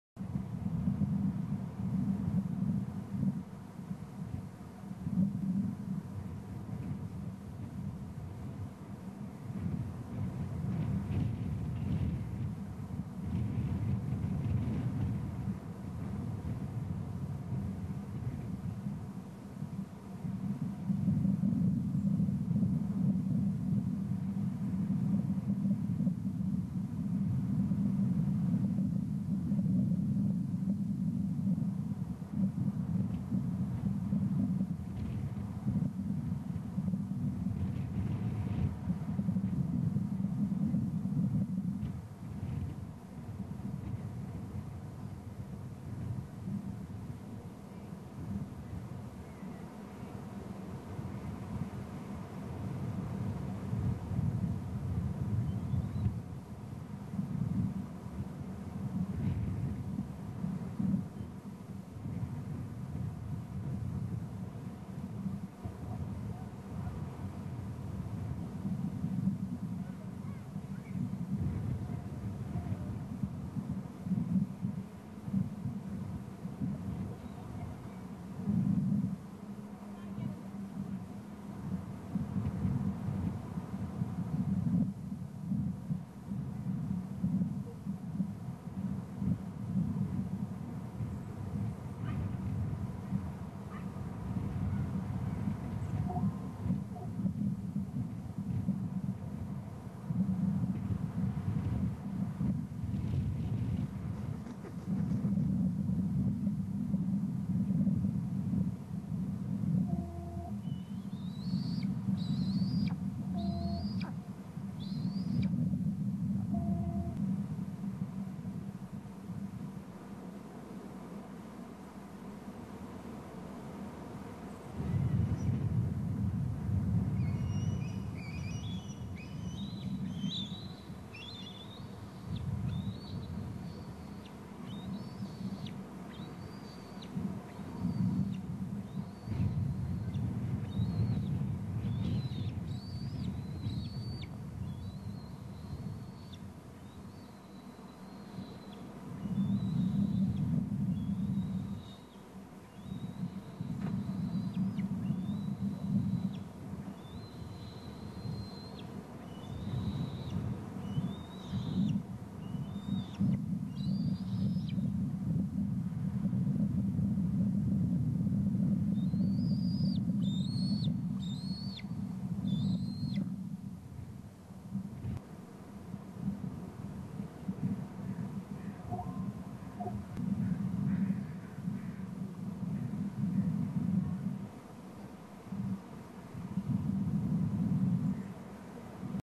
19:20 Em calling 19:23 top eaglet poopshoot 19:25 Em i broken branch ULB of BST
19:20 Em calling single notes 19:23 top eaglet has a huge splooshing poopshoot off the top of topless tree 19:25 cam moves to check for Em; an eagle is on the left little broken branch under ULB of  BST